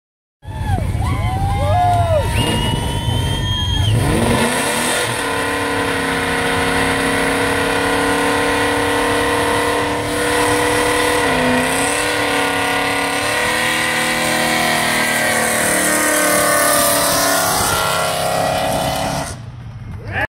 Oak Harbor Apple Festival 2015 car show
Best burnout 67 Malibu super sport